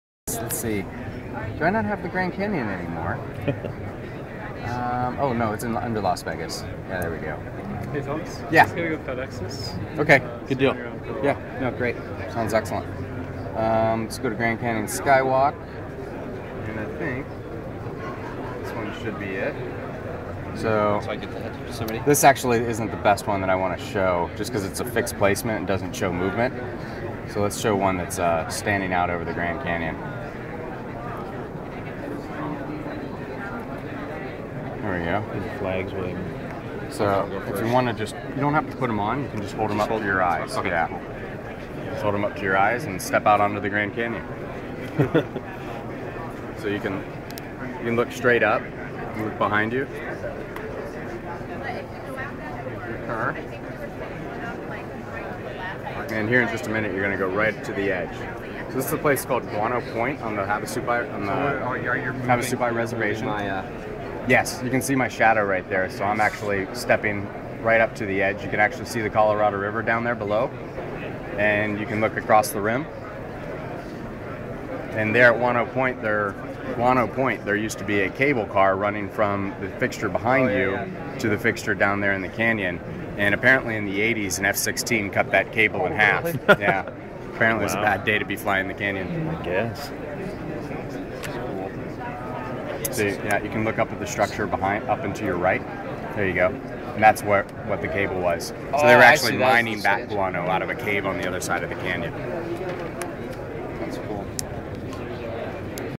Immersive Media Grand Canyon Fly over
At the 2008 ESRI user conference, a demo at the immersive media booth showing some impressive 3D fly-overs of the Grand Canyon. This is the technology that was first used to capture Google Street views 360 at degrees